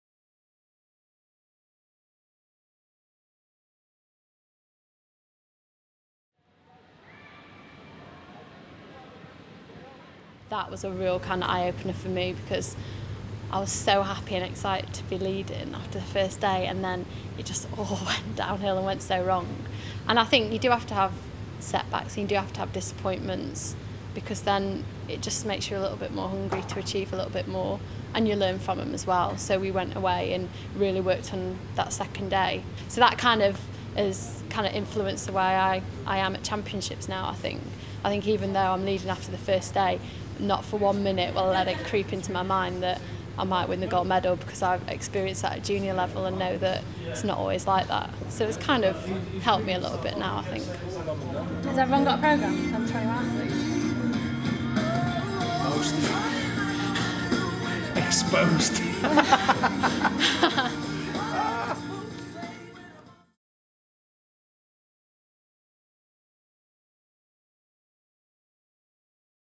Will you be voting for Jessica Ennis for BBC Sports Personality of the Year 2010?
She's World and European Champion, she's the supposed face of London 2012, but will Jessica Ennis be this year's BBC Sports Personality of the Year 2010? It's up to you, so get ready to vote by phone on 19th December. Tune into BBC One from 7pm. Let's help Jess go two better than last year...